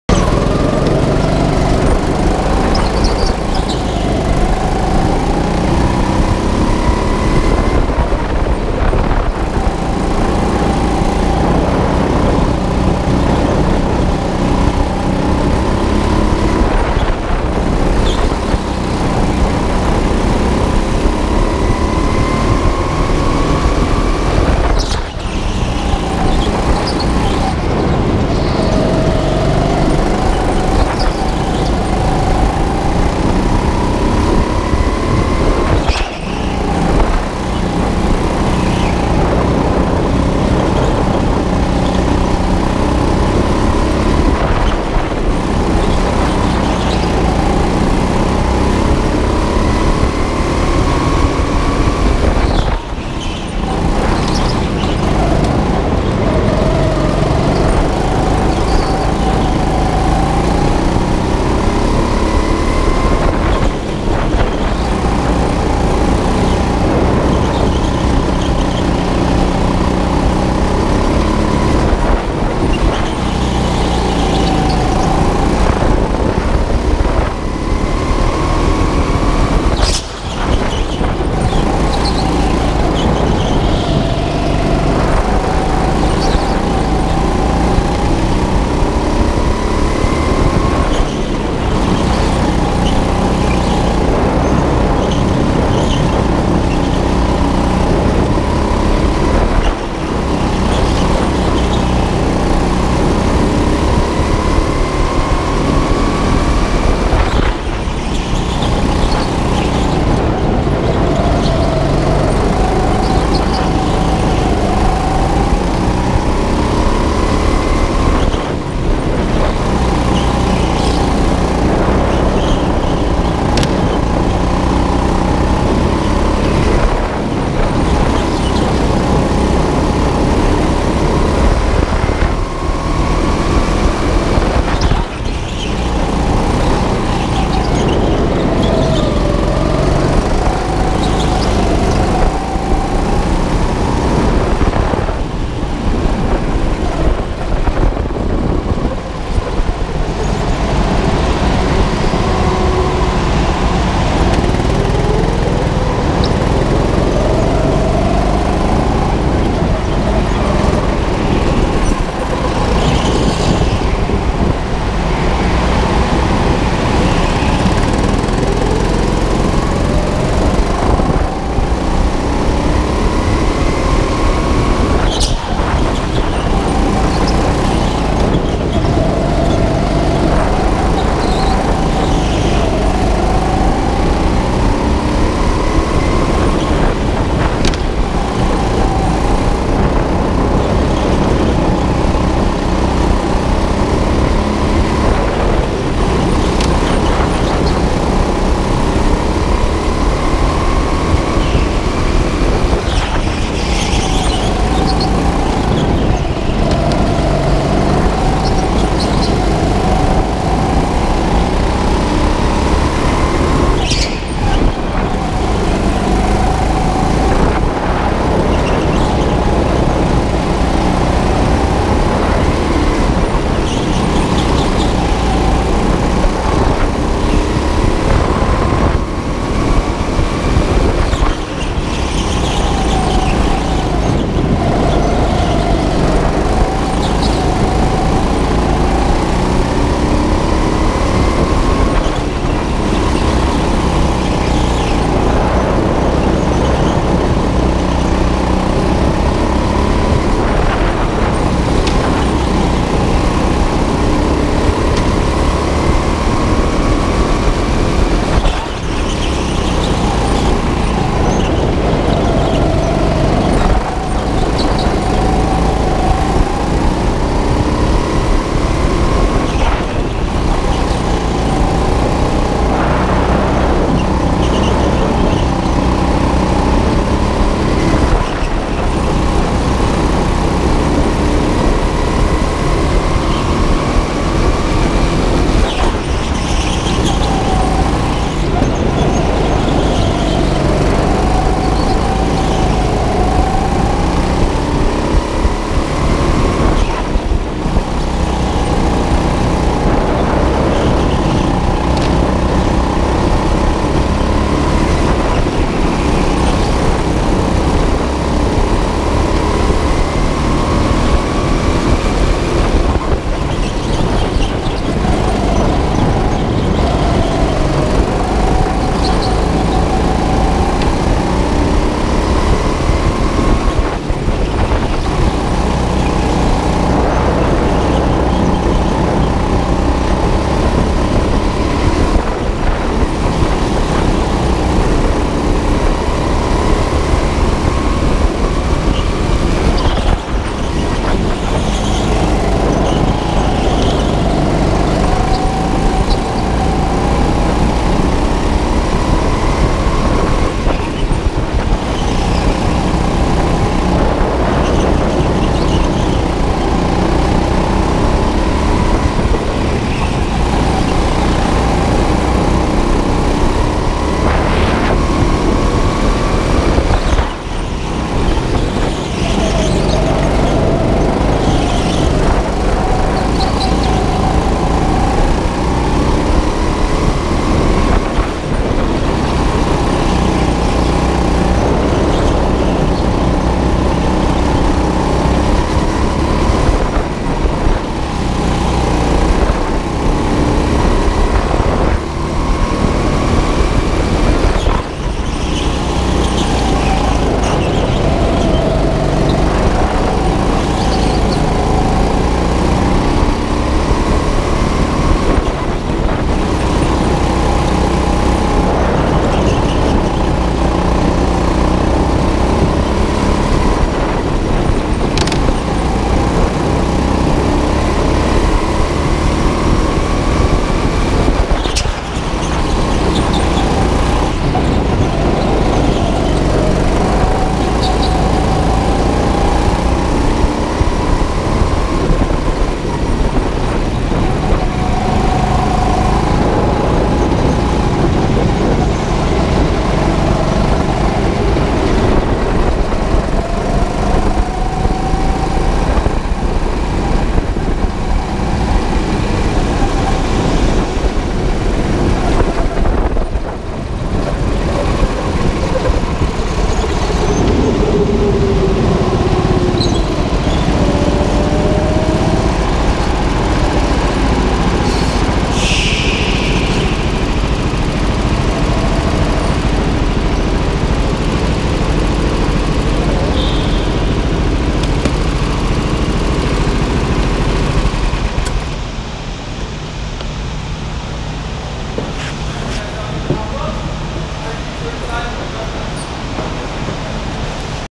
Testing Kart2Kart's New Sodikarts - December 2015
There's still quite a bit of hop from the rear end, but overall they feel pretty good. They ended up being about 7/10ths slower per lap for me around the track, but with less steering effort they're a bit more enjoyable to drive than the last karts.